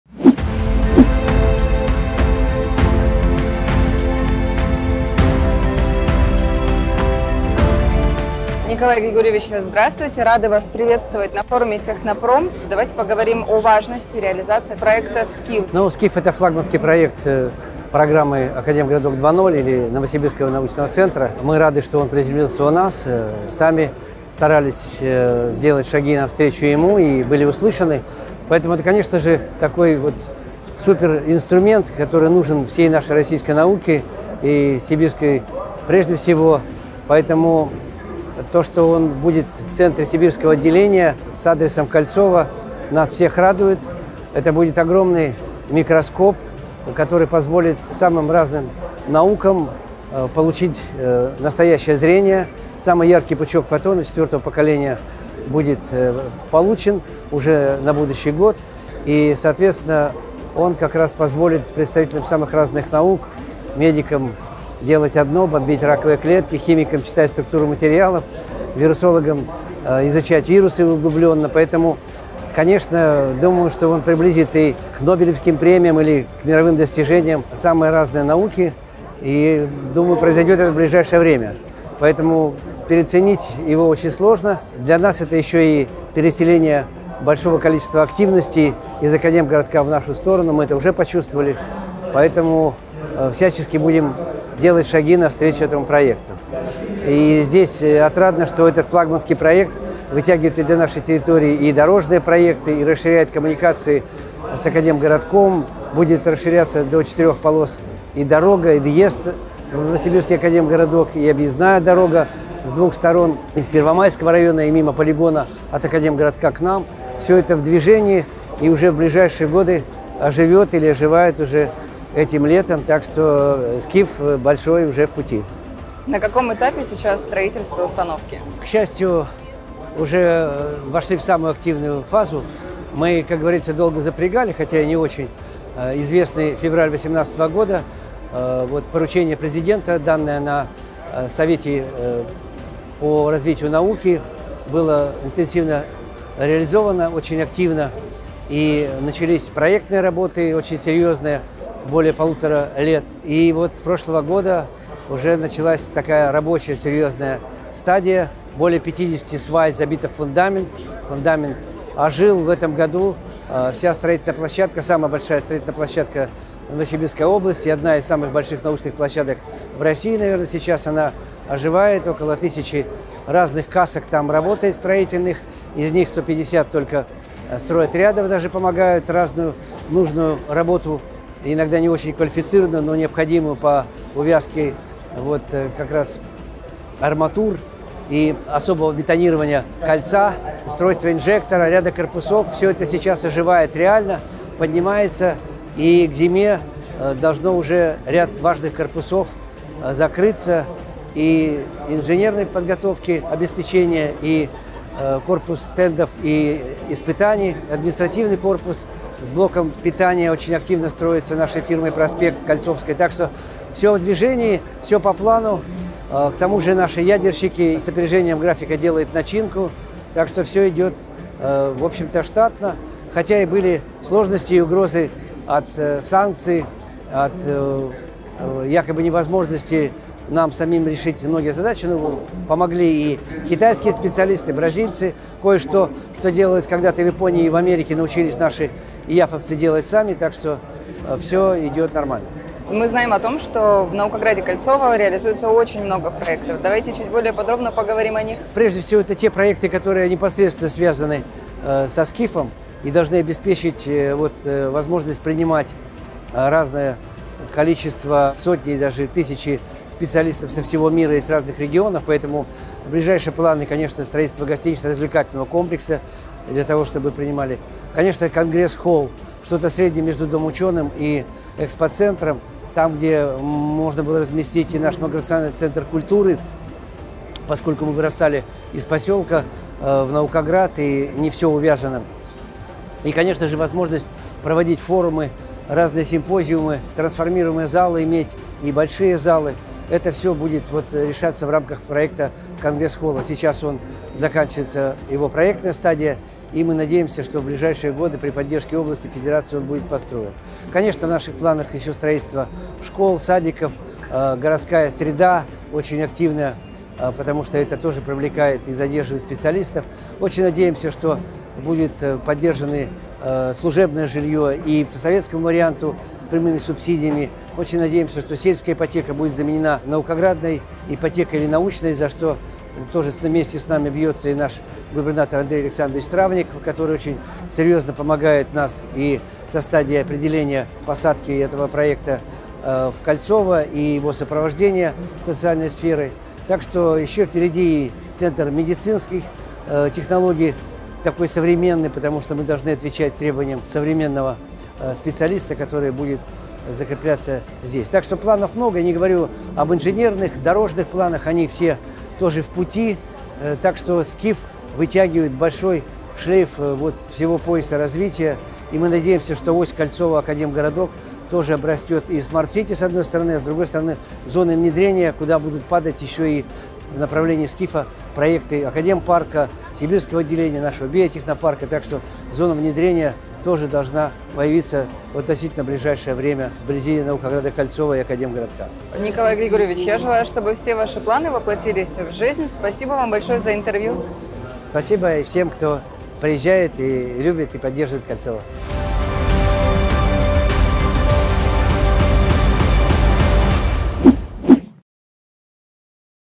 О реализации СКИФ и других проектах в наукограде Кольцово | Событие №47_от 29.08.2023 РБК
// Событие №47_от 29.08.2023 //

Мэр наукограда Кольцово в интервью РБК рассказал о важности реализации проекта «СКИФ», а также о том, что даст строительство Сибирского кольцевого источника фотонов мировому научному сообществу. Кроме того, Николай Красников поделился тем, какие еще проекты планируют запустить в наукограде в ближайшее время.